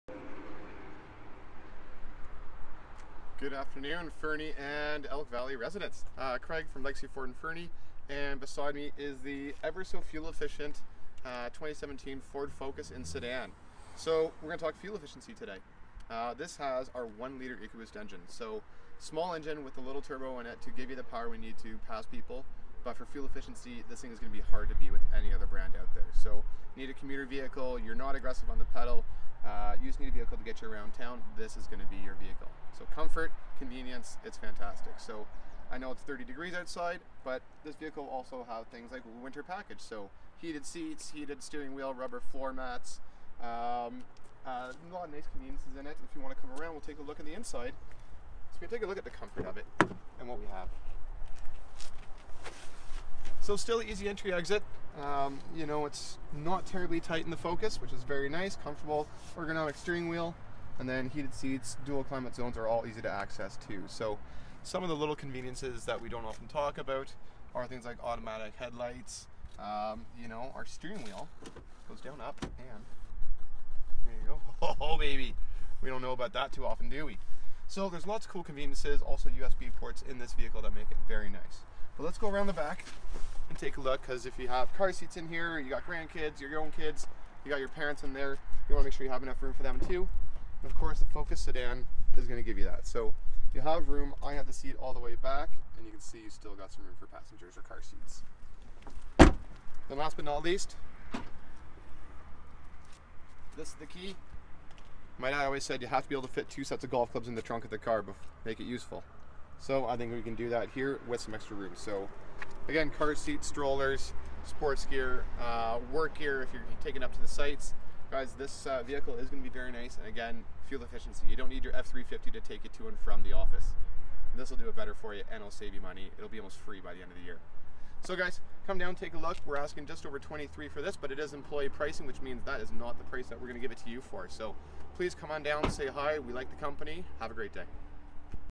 2017 Ford Focus SE | Hot Off the Truck | Fernie, BC
This sleek 2017 Ford Focus is hot off the truck with a sleek magnetic grey exterior and charcoal black cloth interior. With remote start technology and the highly sought-after Sport Package you will receive leather wrapped steering, 17 inch machined aluminum wheels and heated front seats.

Engage yourself in a driving experience that yields fun and excitement every time you get behind the wheel!

Stock # NC0222

Legacy Ford Fernie
6165 Brenners Rd.
Fernie, B.C.V0B 1M5
Canada